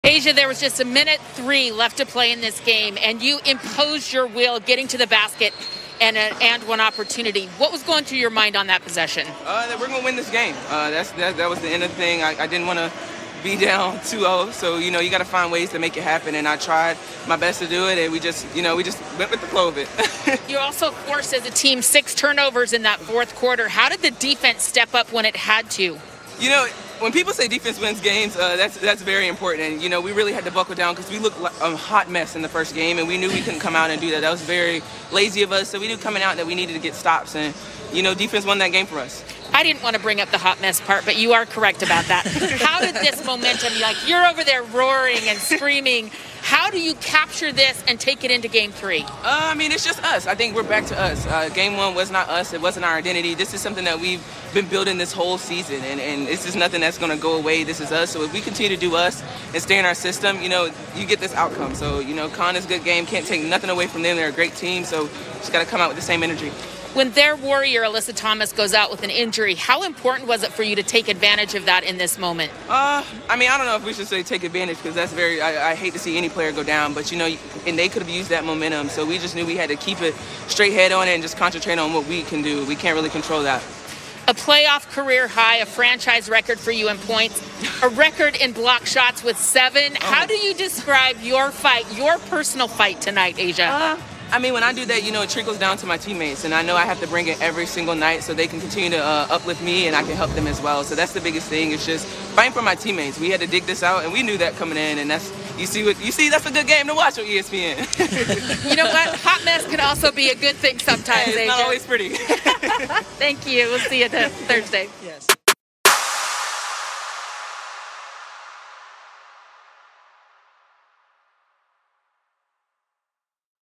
[WNBA Playoffs Semifinals Game 2] A'ja Wilson Post-Game Interview, CON vs LVA, September 22, 2020
WNBA Playoffs Semifinals Game 2 A'ja Wilson Post-Game Interview, Connecticut Sun vs Las Vegas Aces, September 22, 2020